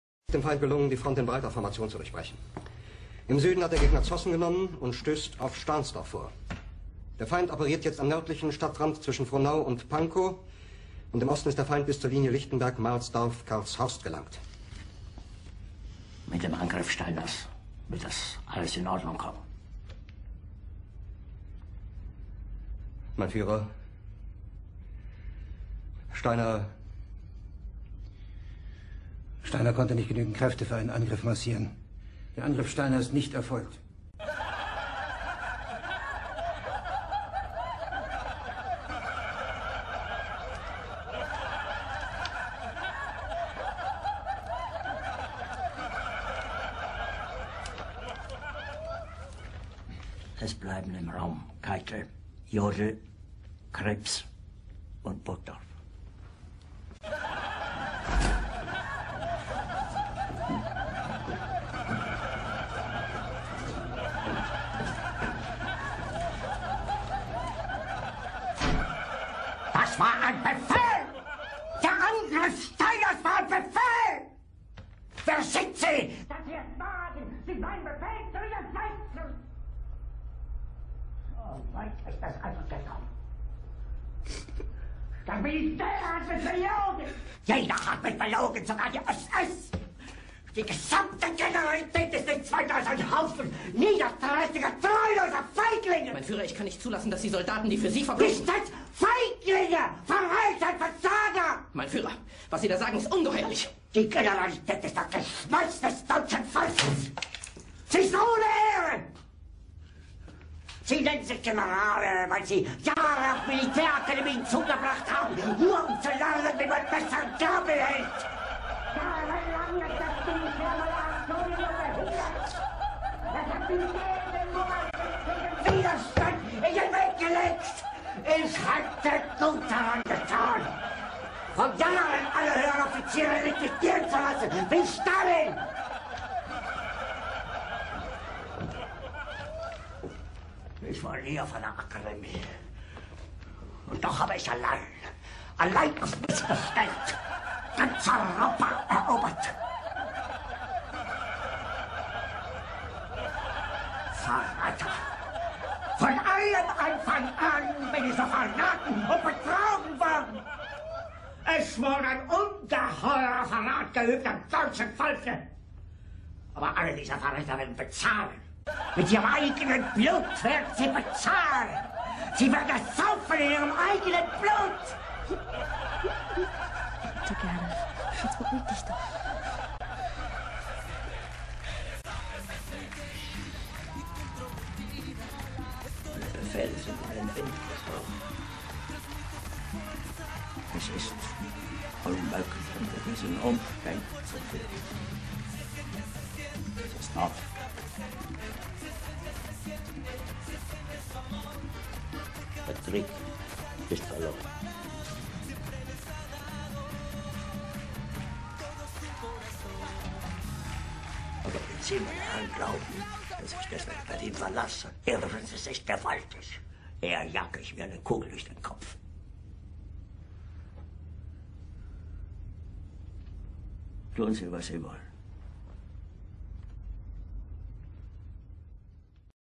hitler se entera de que Laura en America fue censurada en Ecuador
aca hitler se cabrea porque censuraron laura en america en ecuador